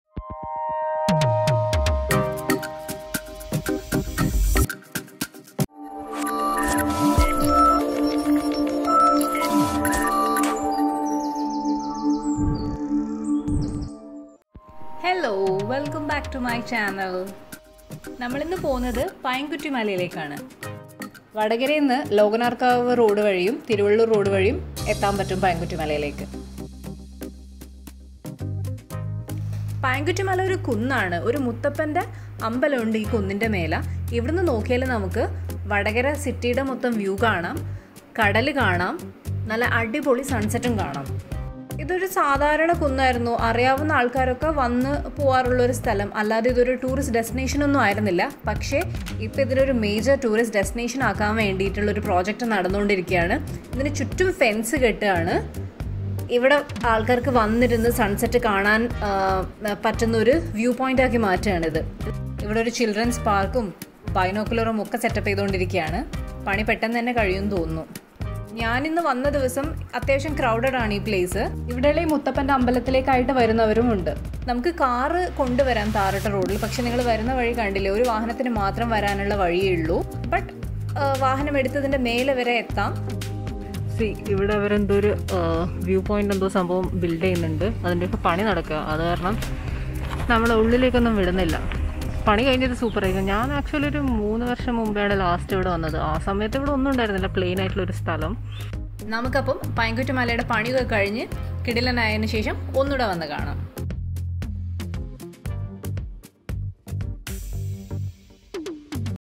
Vadakara Payamkuttimala - Gearing up for a complete makeover‌ | പയംകുറ്റിമല
Payamkuttimala is a hill destination located in the villiappally panchayath of Kozhikode disctrict. A complete make over project for Payamkuttimala is ongoing to develop it as a major tourist zone in North Kerala.

You can reach there through Lokanarkavu road or Vadakara- Tiruvallur road. Payyamkuttimala offers you a panoramic view of Vatakara, its surroundings and Arabian sea. Tourists come here to watch the sunset.
A Muthappan temple is also located on the top of the hill.